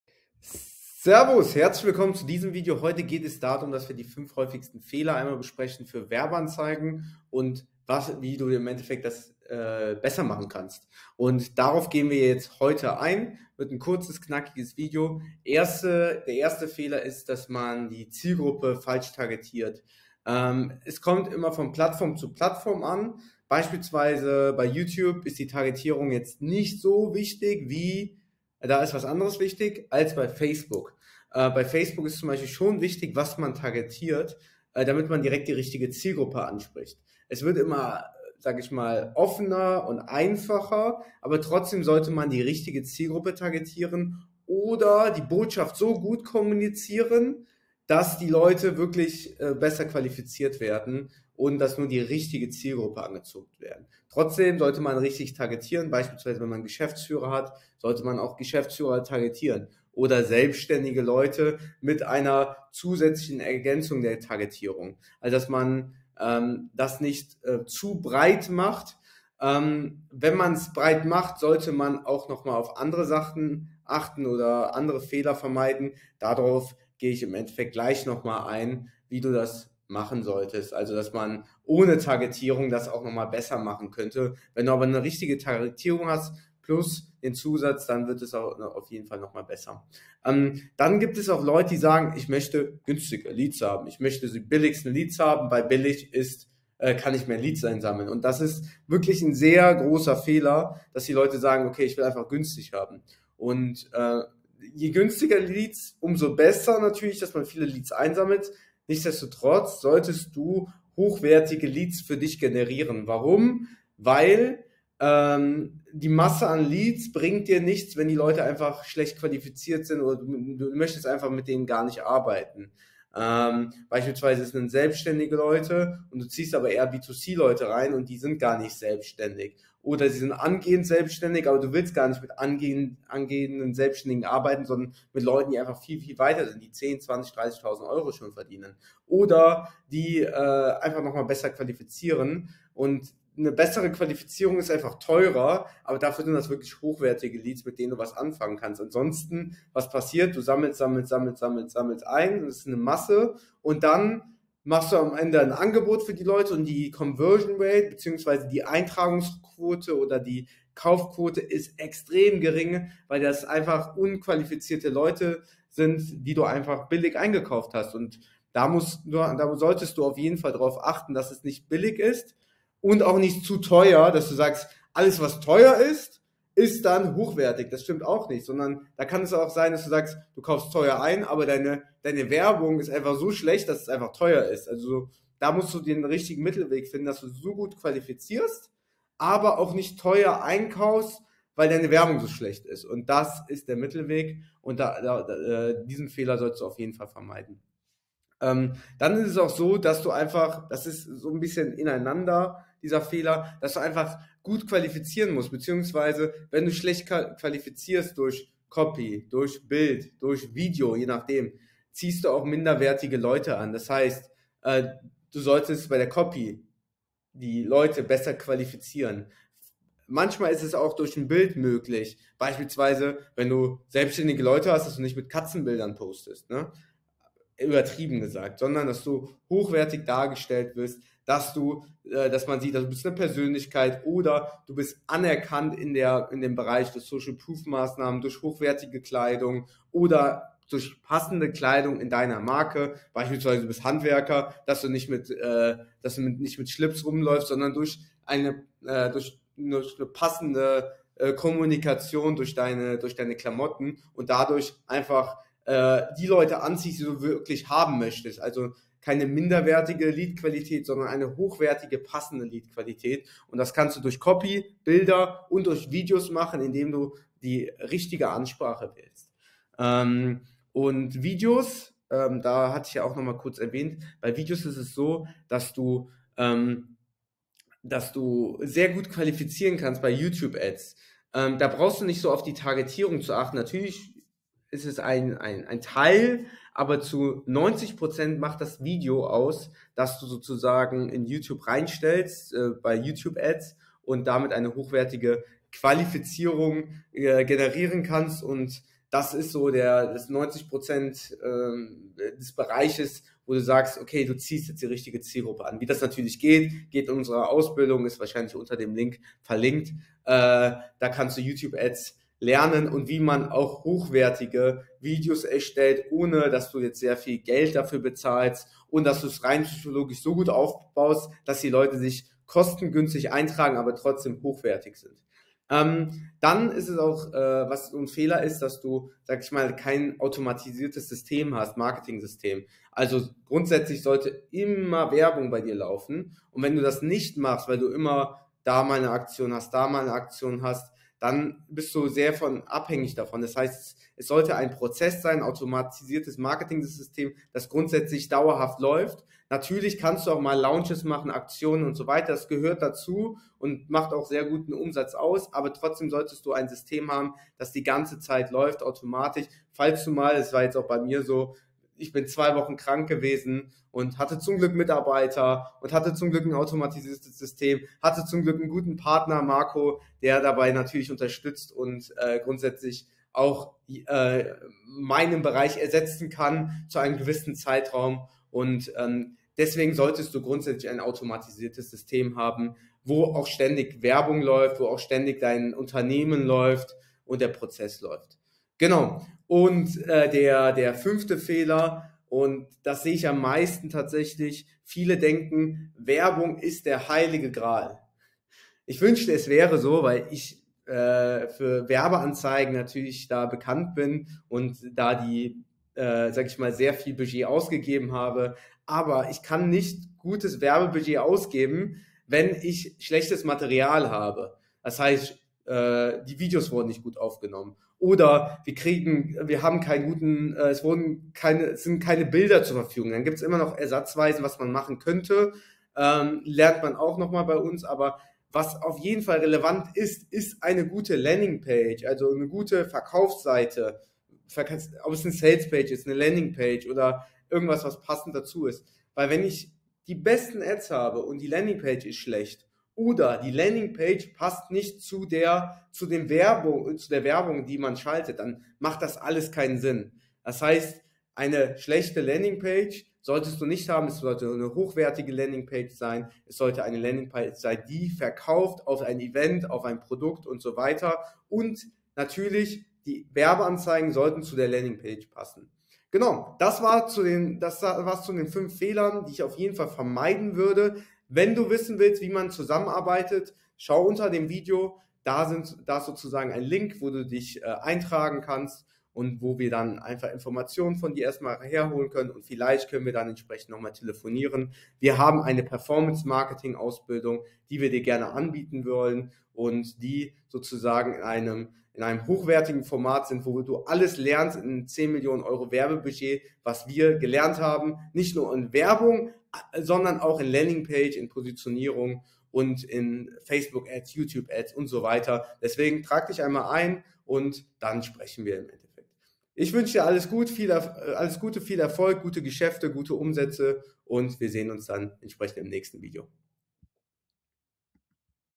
Durch diese 5 Fehler verbrennst du Geld bei Werbeanzeigen!
Viele machen diese 5 Fehler, beim schalten einer Werbeanzeige. Dadurch verbrennst du aber extrem viel Geld.

1. Falsche Zielgruppe (Targetieren)

2. Auf jeden Cent Schauen (“Billige Leads - dadurch billige Zielgruppe.“)

3. Keine Qualifizierung der Ads durch (Copy, Bild, Video) - minderwertige Lead Qualität

4. Kein automatisiertes Marketing-System (Werbung die ständig läuft)

5. Schlechte Landingpage

Schreib uns gerne ein Kommentar und like das Video, wenn es dir gefällt.